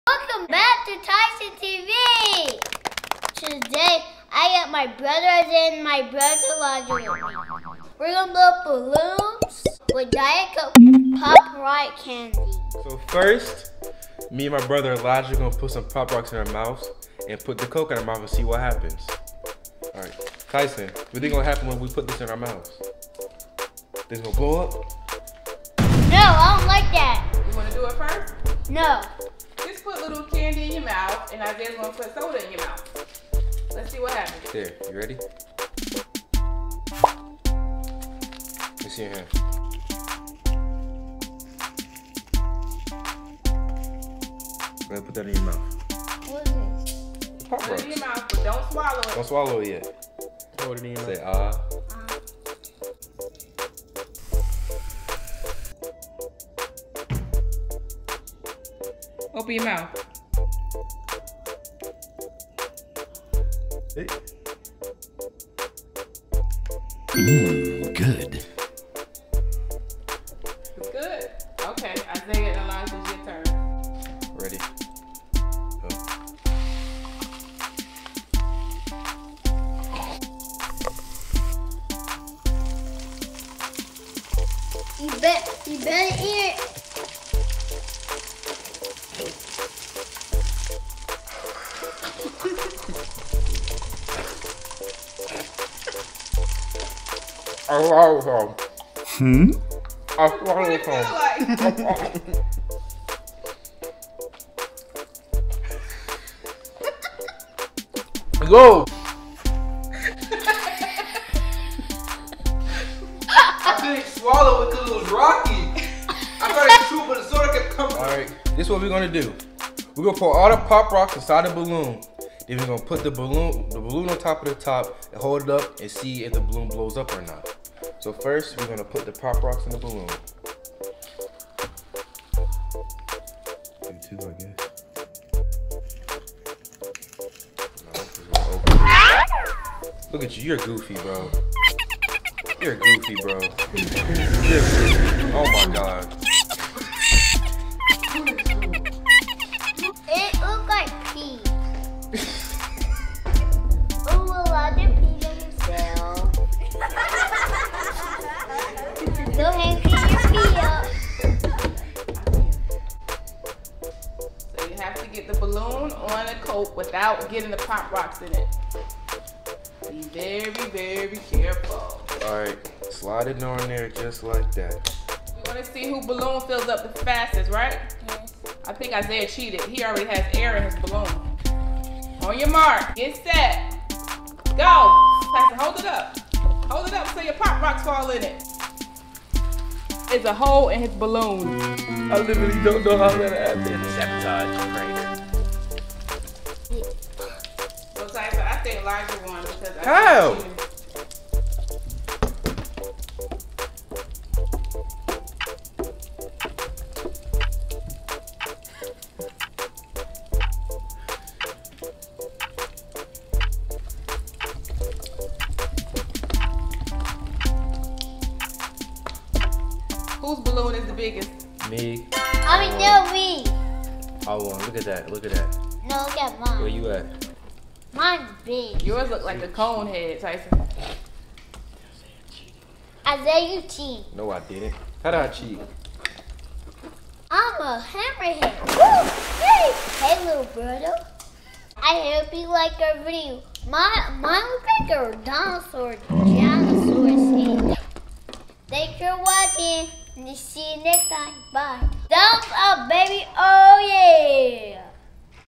INFLATING BALLOONS WITH COKE AND POP ROCKS CANDY. 🎈
Tyson and his brothers inflate balloons using coke and pop rocks candy!